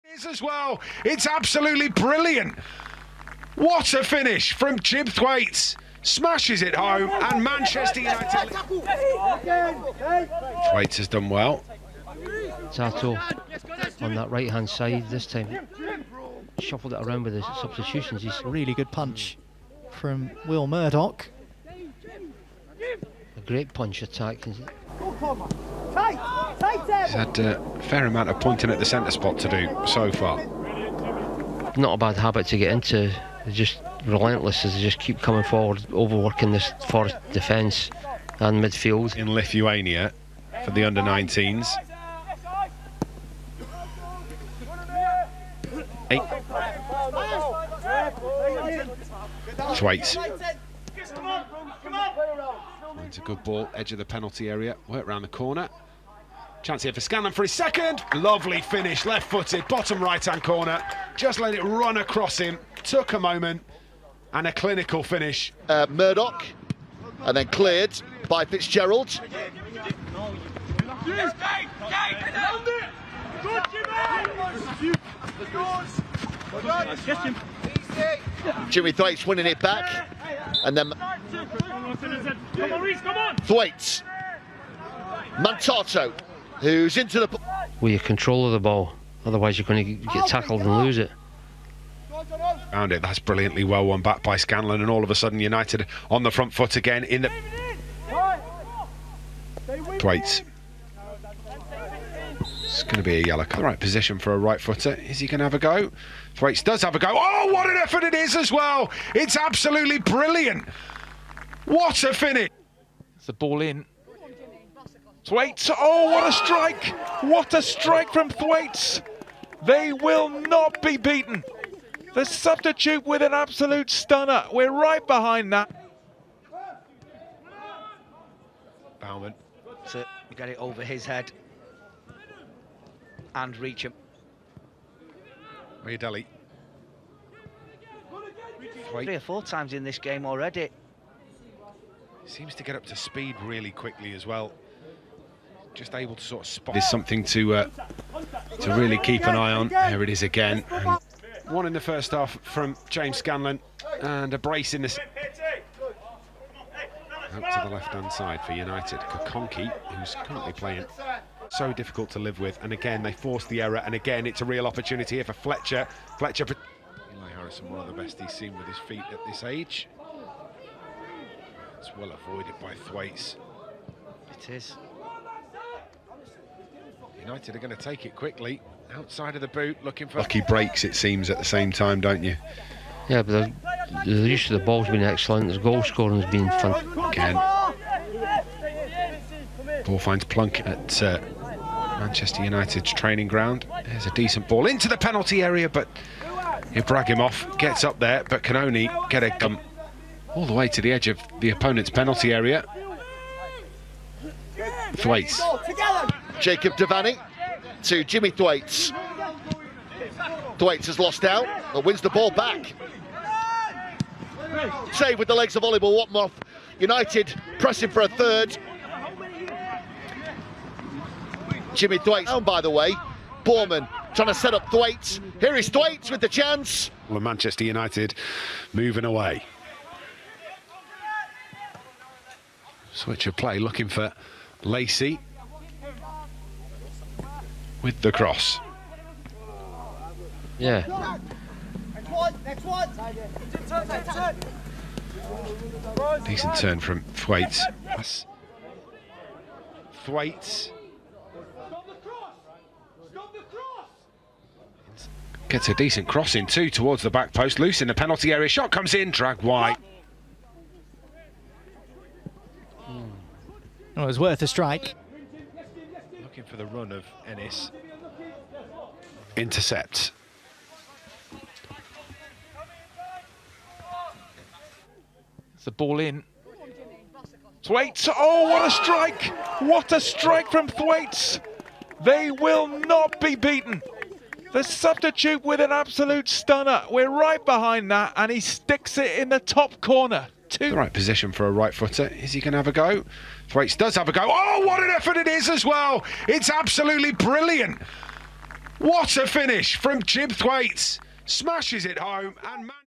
Jim Thwaites is a Baller Wonderkid At Manchester United
Jim Alec Joseph Thwaites is playing as Central Midfielder has been promoted to U18 and U21, Jim Thwaites is a talent for the future, Thwaites promises more to come.
Manchester United wonderkid Jim Thwaites wins Goal of the Season Award in two age groups.